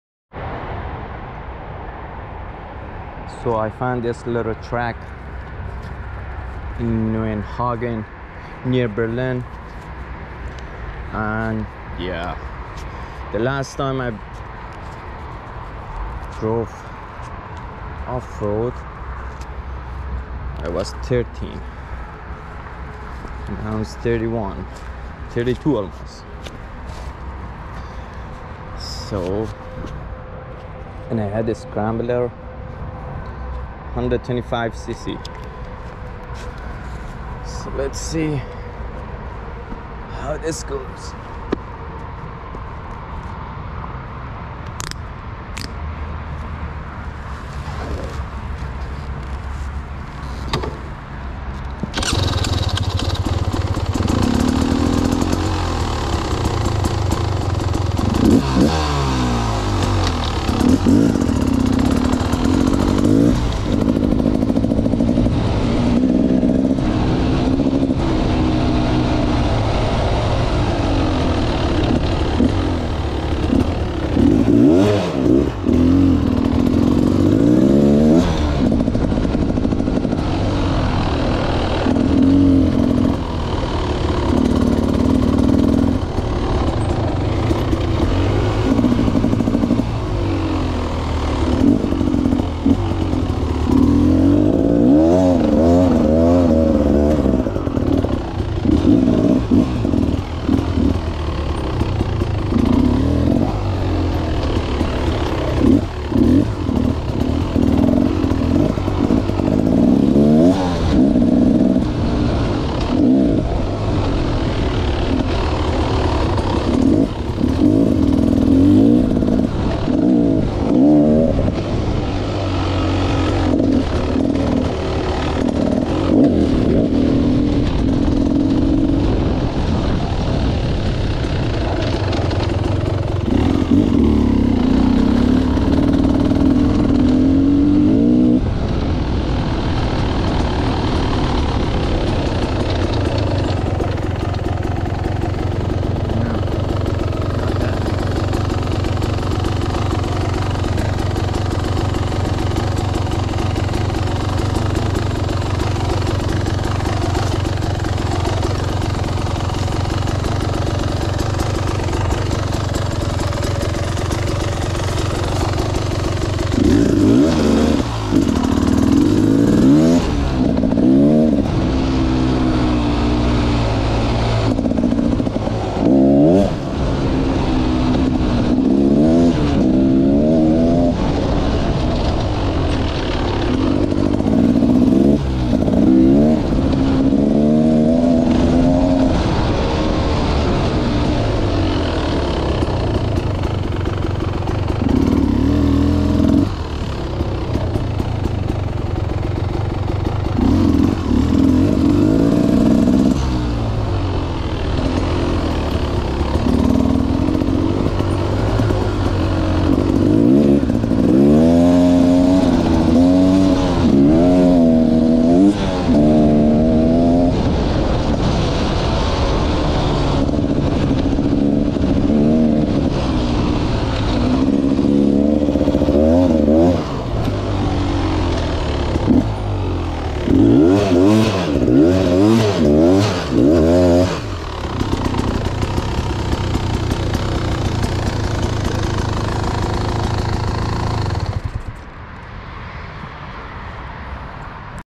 Off-road Track near Berlin - Beta Xtrainer 300
Found a video from two years ago about testing my bike Beta Xtrainer 300 after so many years of not riding motorcycle.